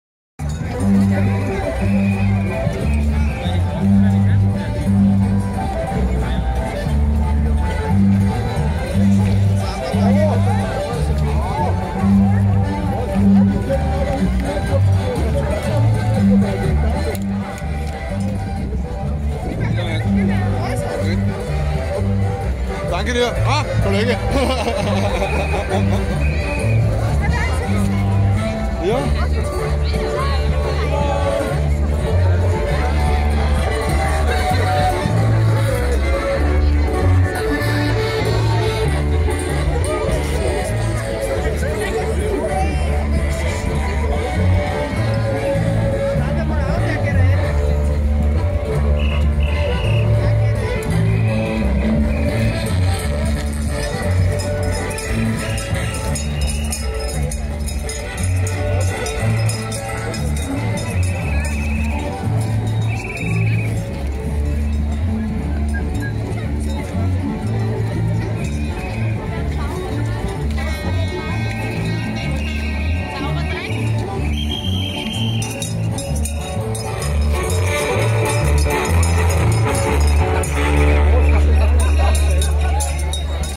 Faschingsgaudi in Laßnitzhöhe - Fasching 2023
Beim Feiern in Laßnitzhöhe wurde getanzt, musiziert und auch für das leibliche Wohl war bestens gesorgt. Sehr zahlreich strömten die Narren nach Laßnitzhöhe zur Fachingsgaudi, zu der die Trachtenkapelle Laßnitzhöhe am 19. Februar 2023 geladen hatte.